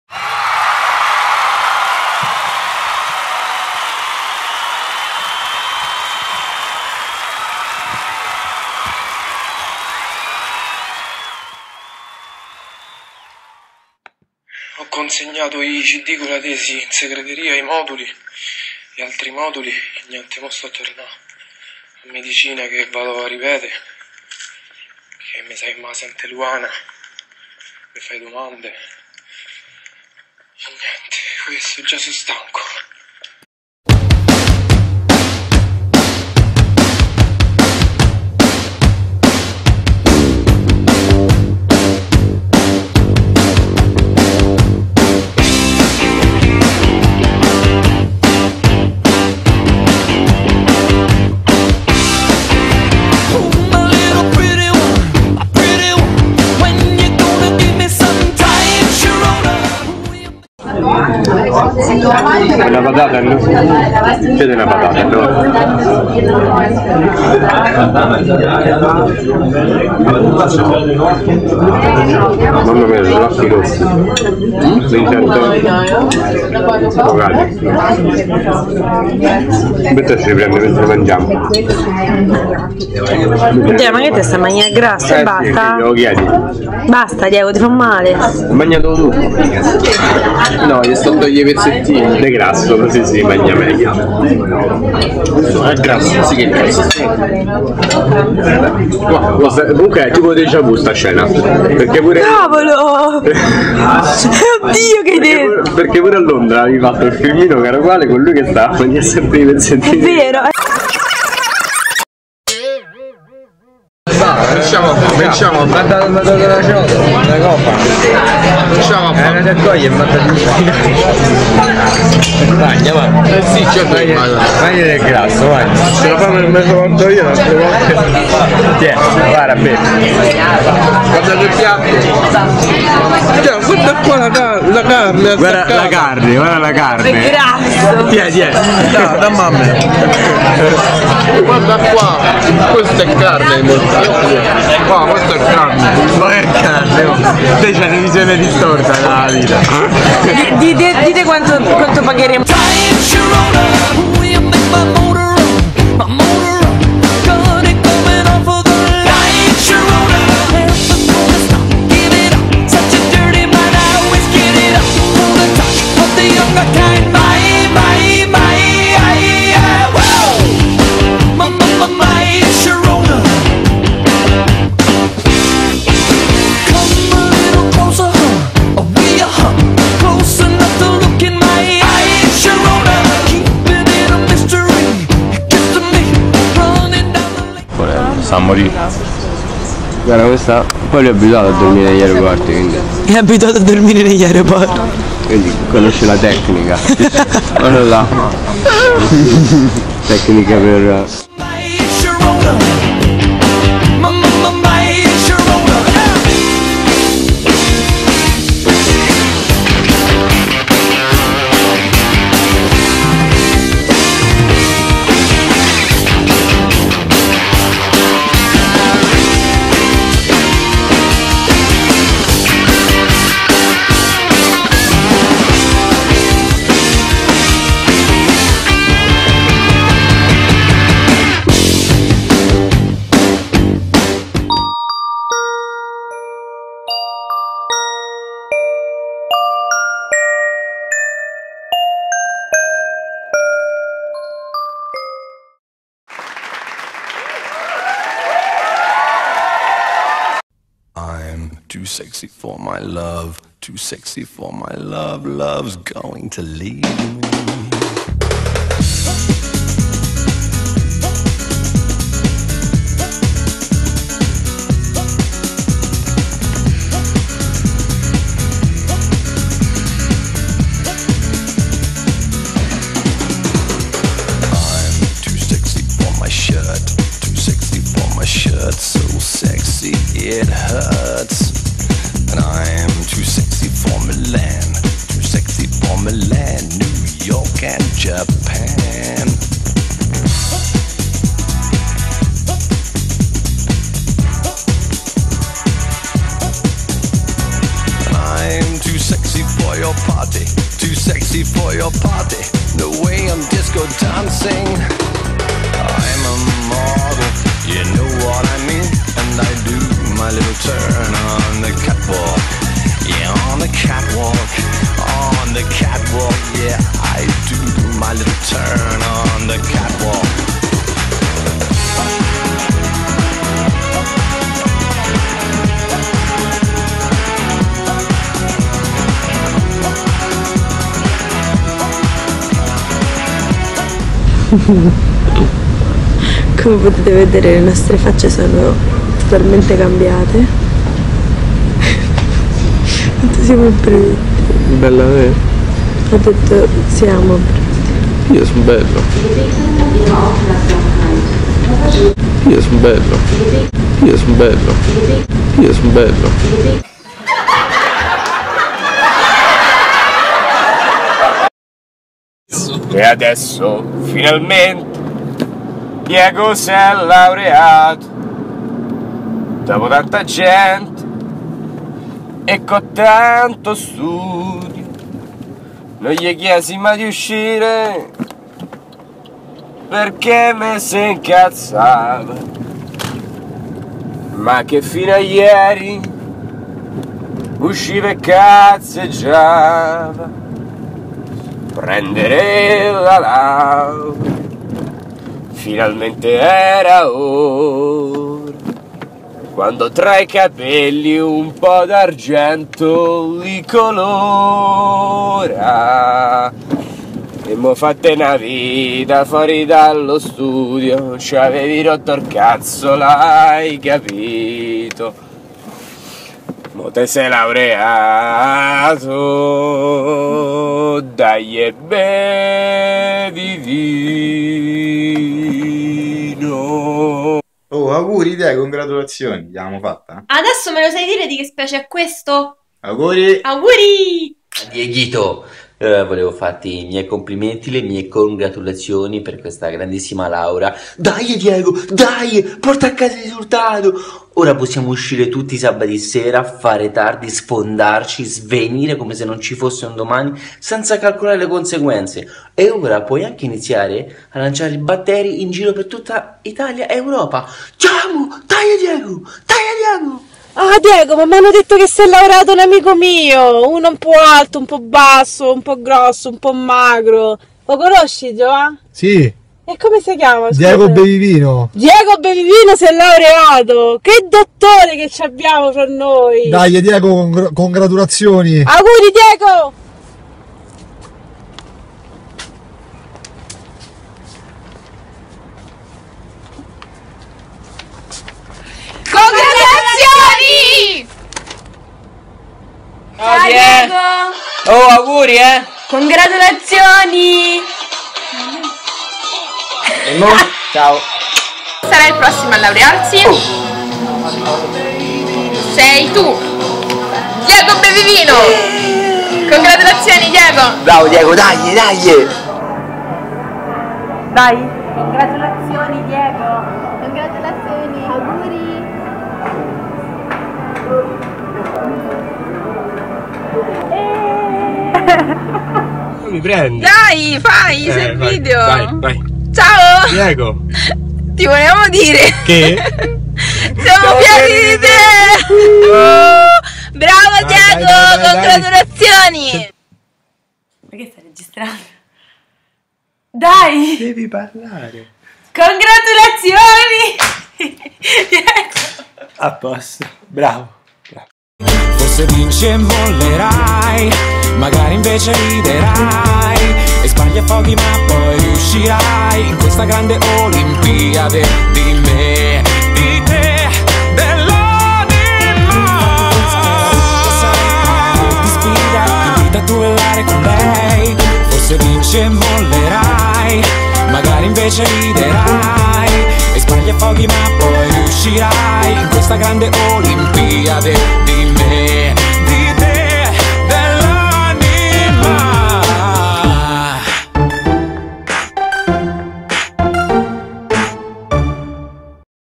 Come Nasce un Dottore
Laurea Diego, 27 maggio 2015. Regalo da parte degli amici